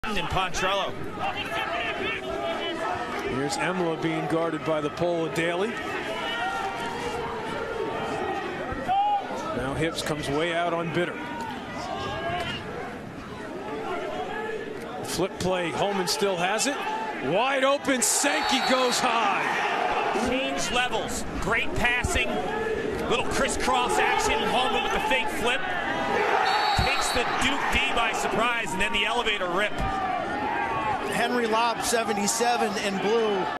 UNC 3-man weave and fake flip leads to Sankey goal
North Carolina runs a 3-man weave up top with Jimmy Bitter #4, passing to Marcus Holman #1 who fakes a flip to Chad Tutton #12 and then feeding Joey Sankey #11 for a goal while Duke defender Henry Lobb #77 thinks Tutton has the ball.